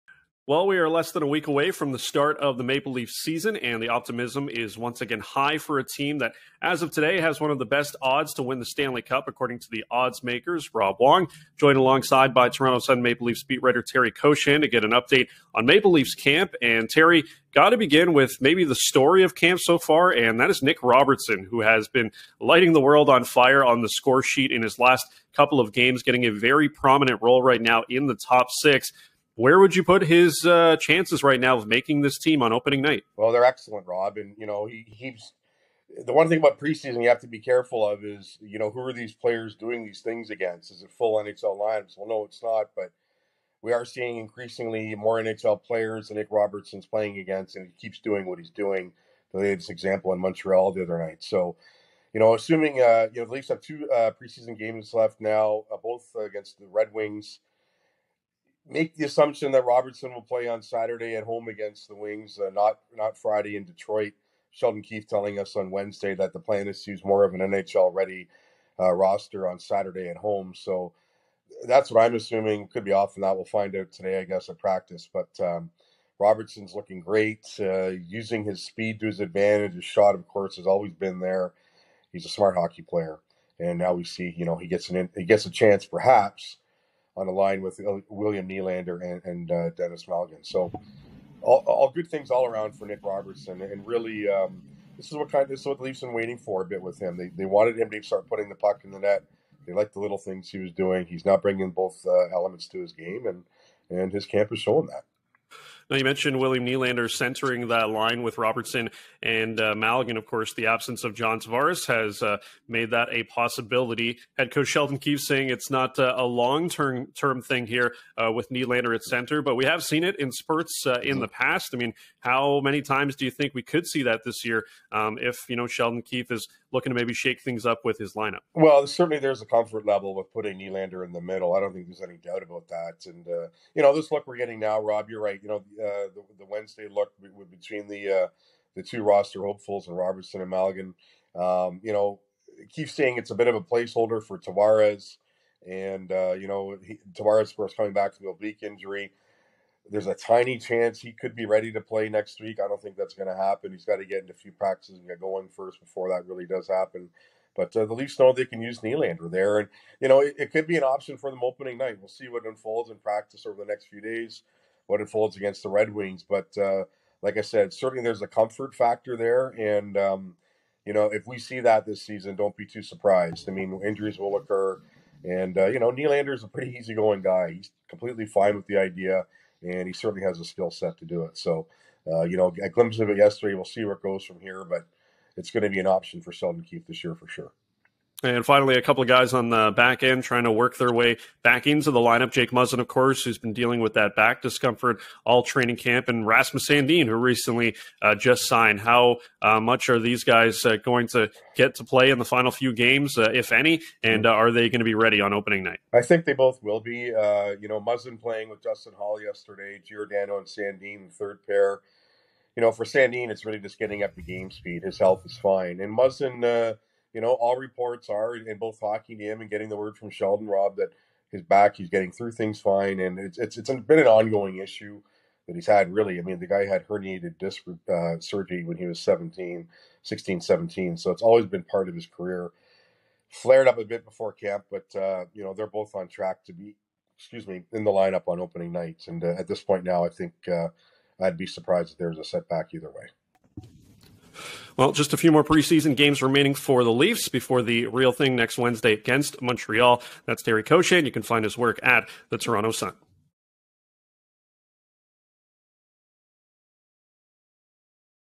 LEAFS TRAINING CAMP: Robertson ready for his shot
Postmedia's Rob Wong and Toronto Sun Maple Leafs writer Terry Koshan chat about Nick Robertson's impressive preseason play, William Nylander playing centre and whether or not Jake Muzzin and Rasmus Sandin will be ready for the regular season.